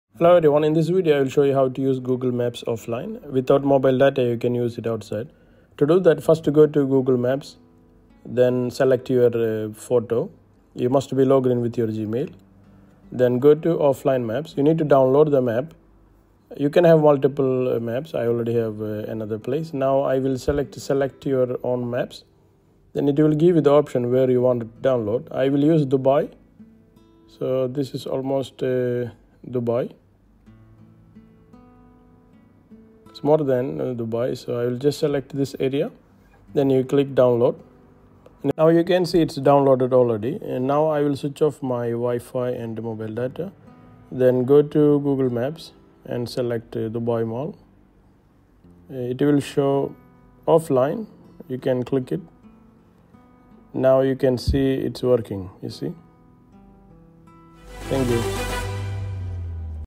Use Google Map Offline without Mobile Data.
✅ Step-by-Step: Use Google Maps Offline
🧭 Step 1: Open Google Maps App
Open the Google Maps app on your Android or iOS device.

📍 Step 2: Search Your Area
In the search bar, type the name of the place (e.g., "London", "New York", "Paris").

Once the location appears, tap on it.

⬇️ Step 3: Download the Map
After selecting the location, tap the three-dot menu (top right corner on Android or below the location name on iOS).

Tap "Download offline map" or "Download".

✅ You may need to adjust the area you want to download by zooming in/out.

☁️ Step 4: Wait for Download
Google Maps will start downloading the selected area.

Ensure you're on Wi-Fi to save mobile data.

🔌 Step 5: Turn Off Mobile Data (To Test)
Go to your phone's settings and turn off mobile data or Wi-Fi.

Open Google Maps – the downloaded area will still work for:

📍 GPS navigation
🗺️ Viewing streets and locations
🚗 Driving directions (no transit/walking)

🚫 What You Can't Do Offline
- No live traffic updates
- No transit (bus/train) directions
- No route alternatives
- No real-time ETA updates

🛠️ Managing Offline Maps
Go to Google Maps → Tap your profile picture → Offline maps

From here, you can:
--Rename maps
- Update them
- Delete them